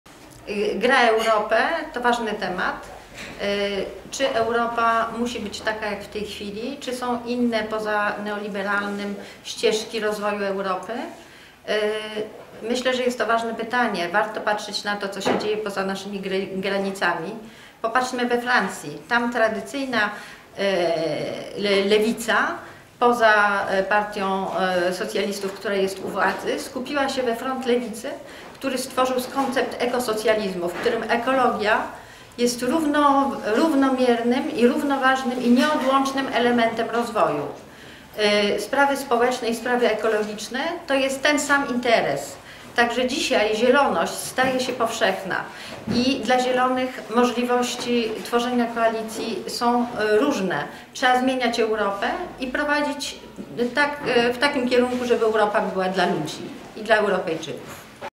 Gra o Europę: Ewa Sufin-Jacqumart o ekosocjalizmie
Czy Europa skazana jest na neoliberalny kierunek dalszej integracji? A może istnieje alternatywa? Jakich sojuszników powinni szukać europejscy Zieloni dla swojego projektu innej Europy?
Ewa Sufin-Jacquemart o „Grze o Europę" -- książce i cyklu debat. Więcej informacji oraz